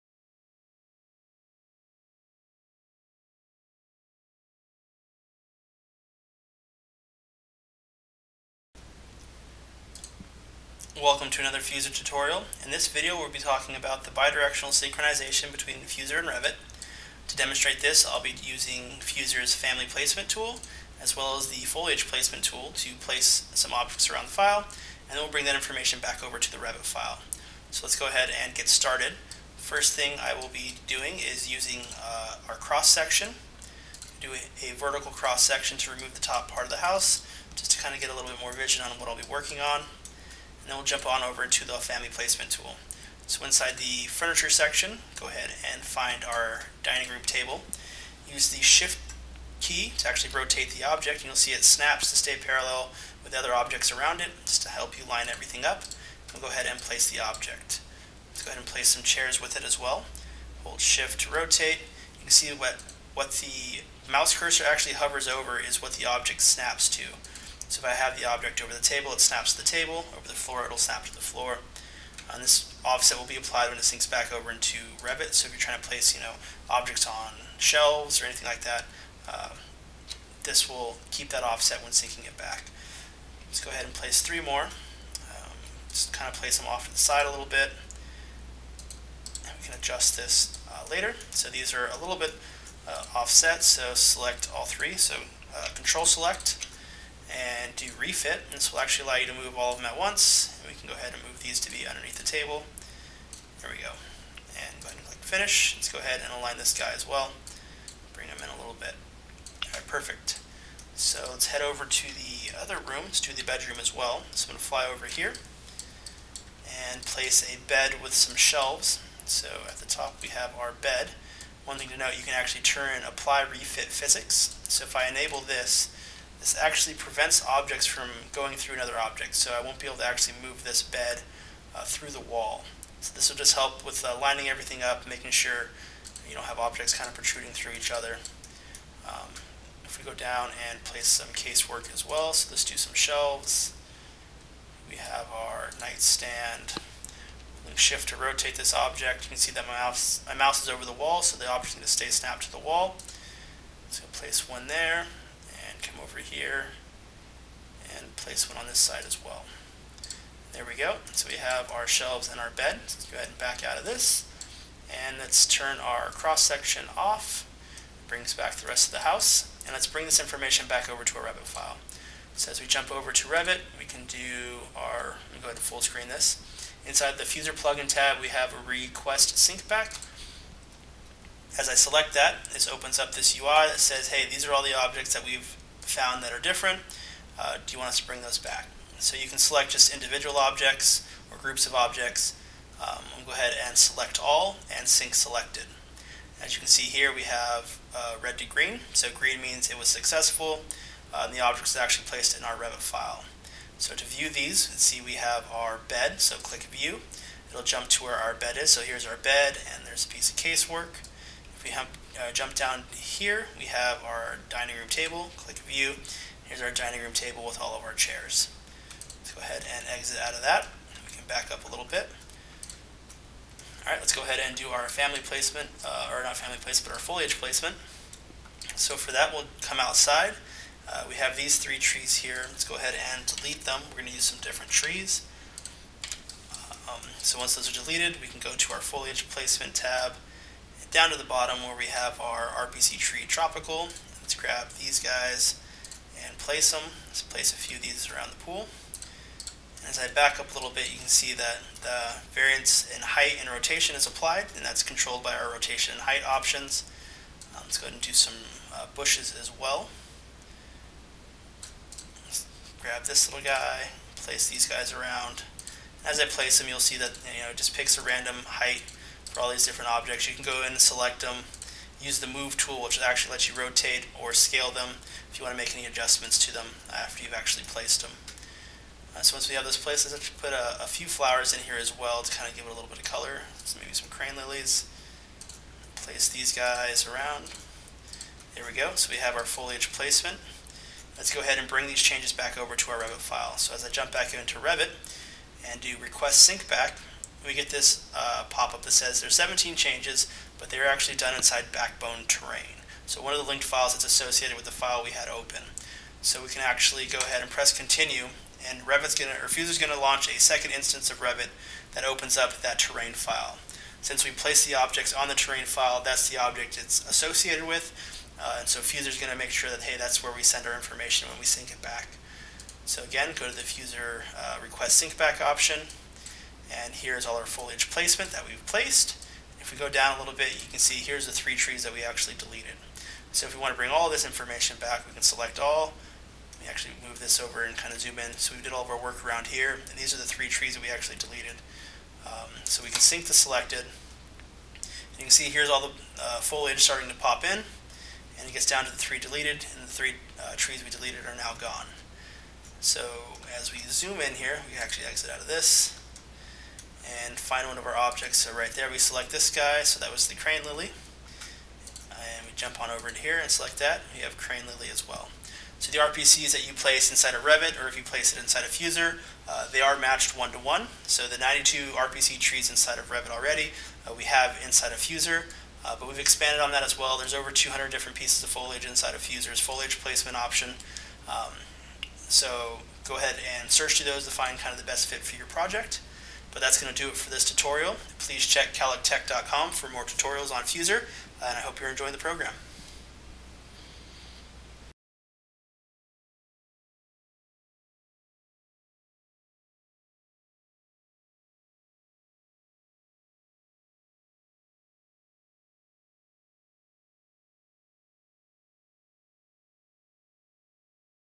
Bidirectional Synchronization
Fuzor tutorial video for using the bidirectional synchronization with family placement and foliage placement.